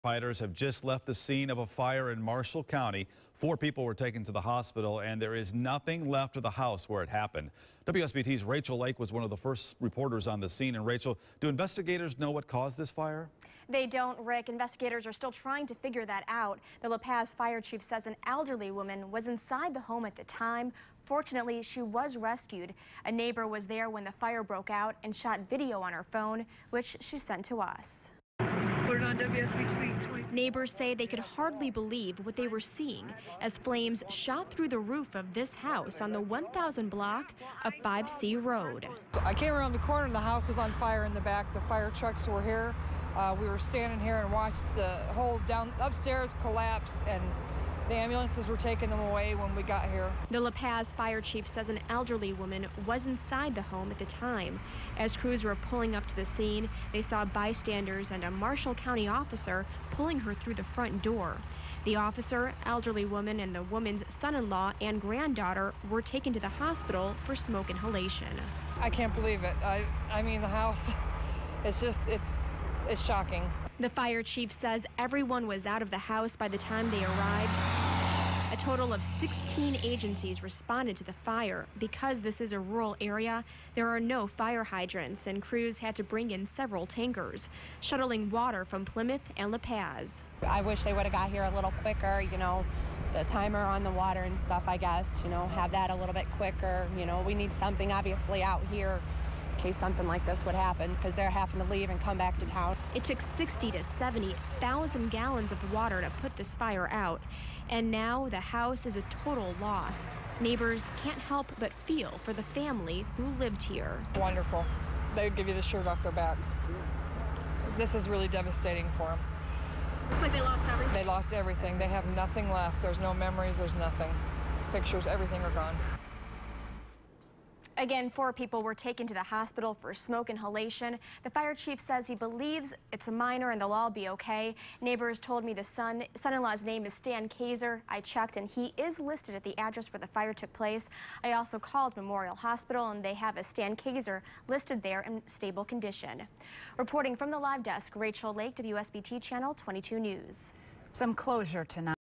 Fire destroys Marshall Co. house
Investigators are trying to determine what caused a home near LaPaz to burn down Tuesday. Four people were taken to the hospital, including three family members inside the home and a county police officer who helped save them. WSBT's Rachel Lake has the latest. _________________